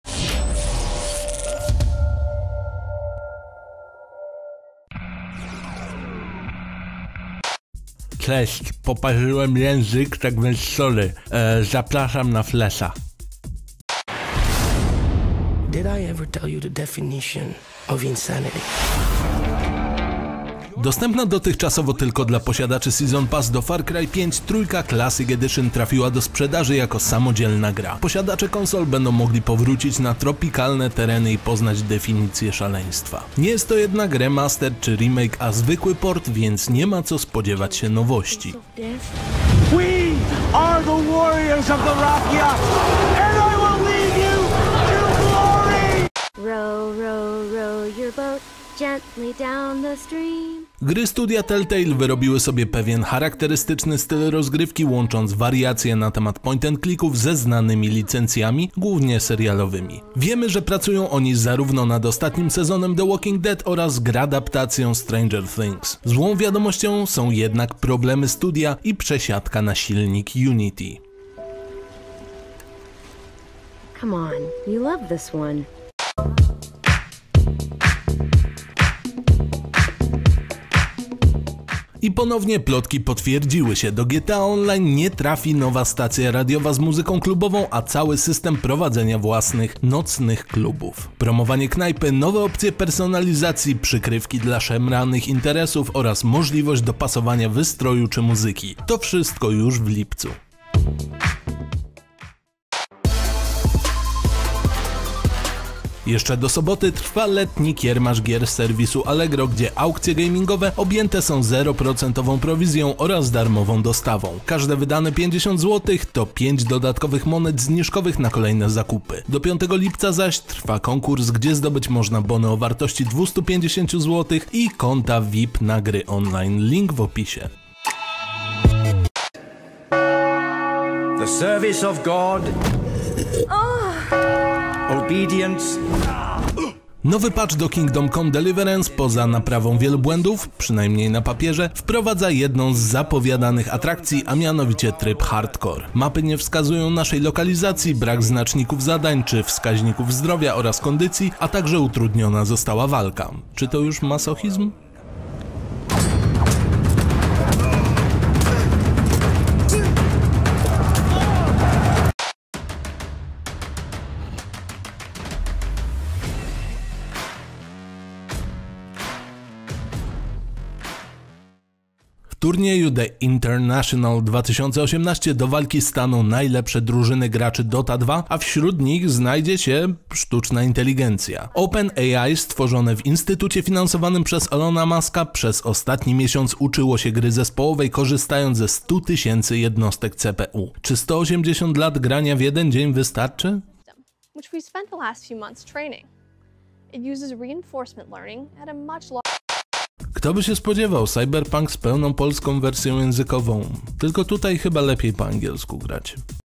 GTA Online otrzyma kluby nocne. FLESZ – 26 czerwca 2018
GTA Online z systemem prowadzenia nocnego klubu? Takie atrakcje ma zaserwować Rockstar w lipcu. We Fleszu mówimy też o nowym silniku dla gier studia Telltale, powrocie Far Cry 3, trybie hardcore do Kingdom Come oraz sztucznej inteligencji, która zagra w DOTĘ 2.